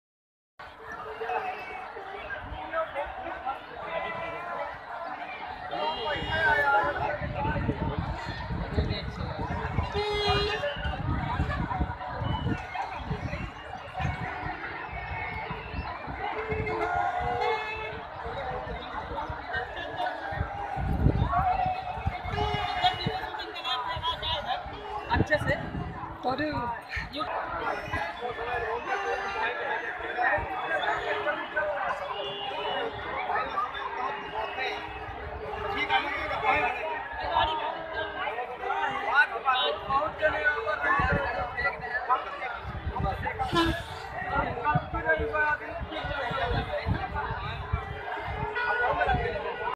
Indian village Mela festivals girl and boyes more entertainment bheed Todayvideos
Indian village Mela festivals girl and boyes more entertainment bheed

hello friends.
Village fair is very common in India. Thousands of people visit these fair, these fair are held generally on the occasion of some religious festivals.